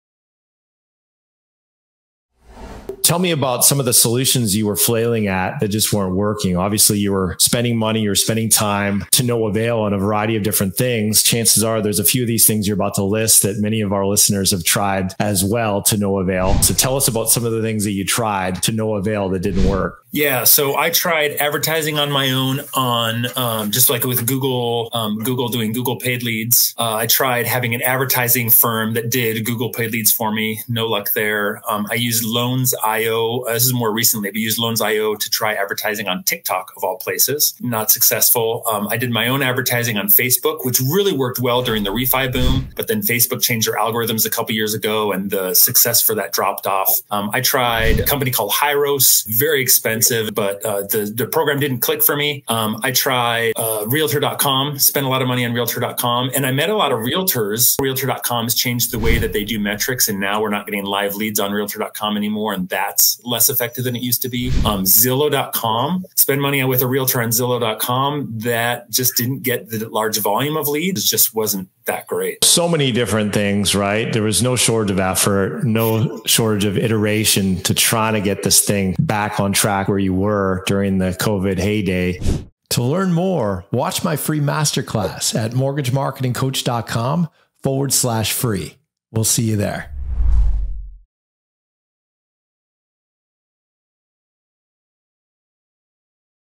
Ben Davis - Part 1
CASE STUDY (Part 1 of 6): Trying lots of different marketing methods to no avail? Ben Davis suffered the same plight in frustration and defeat, and then he finally broke through and found a way to 5X his closings in just 6 months - without making a single cold call. 🚀 Here’s how…